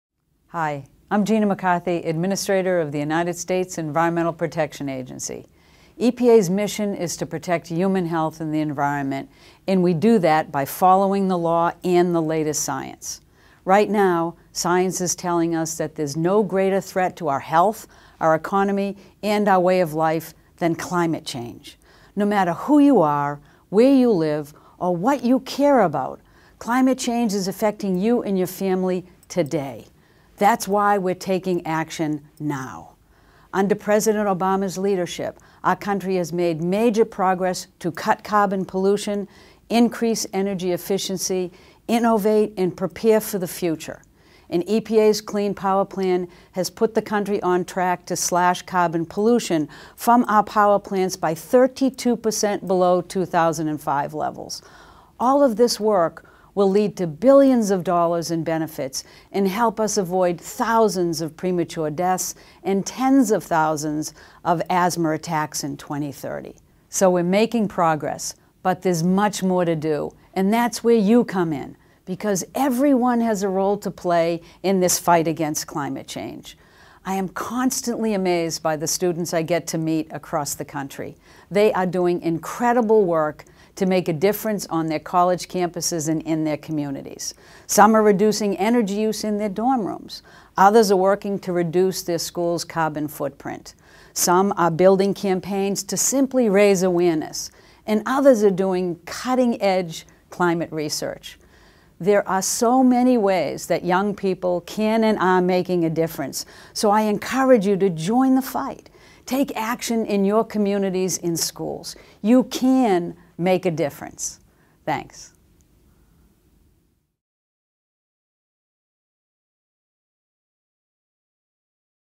EPA Administrator Greeting: Know Tomorrow Day of Action
Description:  Know Tomorrow is a student-led event to educate communities on how to take action on climate change. Students from more than 50 schools across the country come together for a  Day of Action with campus events, rallies and concerts.

For more information about the work EPA is doing on climate change, go